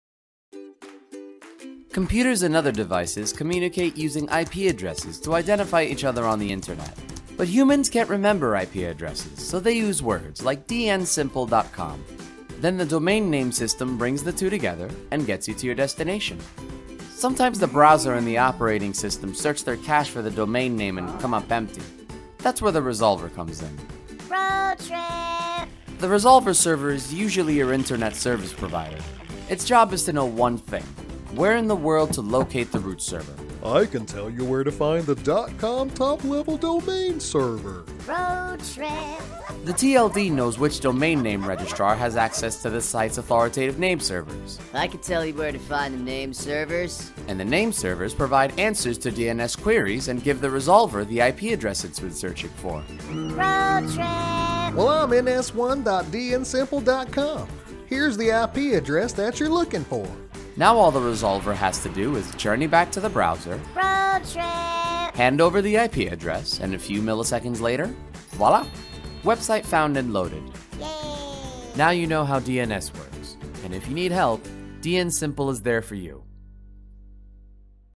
What is DNS? How a DNS Server (Domain Name System) works? Learn about How DNS Works with our video!
What is DNS? We created a fun, colorful explanation of how DNS works. Have you ever wondered how computers and other devices communicate using IP addresses? In How DNS Works, we take a road trip from the Resolver to the Root server and back to the browser to show what happens when you type a URL in your browser, we explain all the difficult notion like of authoritative name, domain server, name server and everything around DNS . Enjoy!

We help set you up for success with our worry-free DNS, SSL & Domain Management.

⏰ Time Stamps ⏰
00:00 The domain name system brings together IP addresses and words we can understand
00:17 The resolvers one job: locating the root server
00:39 The TLD helps you find the right name server
00:54 ns1.dnsimple.com communicates the correct IP address
01:06 The resolver communicates the given IP address to the browser
01:11 Success!
01:15 DNSimple is here for you